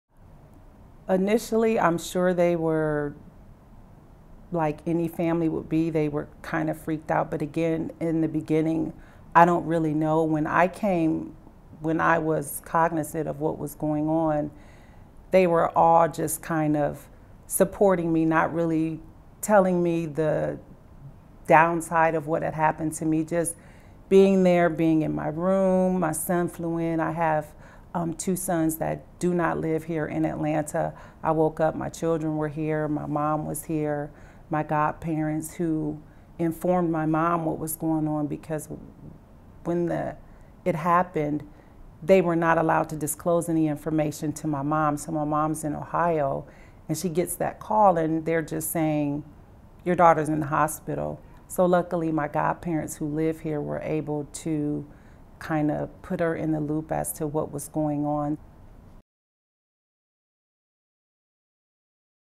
Jabree: How did family members respond to the news?
Injured in 2015 at age 42, Paraplegic

Initially, I’m sure they were like any family would be, they were kind of freaked out, but again in the beginning, I don’t really know. When I came, when I was cognizant of what was going on, they were all just kind of supporting me, not really telling me the downside of what had happened to me, just being there, being in my room. My son flew in, I have two sons that do not live here in Atlanta, I woke up my children were here, my mom was here. My godparents, who informed my mom what was going on because when it happened, they were not allowed to disclose any information to my mom. So, my mom is in Ohio, and she gets that call, and they’re just saying, “your daughter is in the hospital.” So, luckily my godparents, who live here, were able to kind of put her in the loop as to what was going on.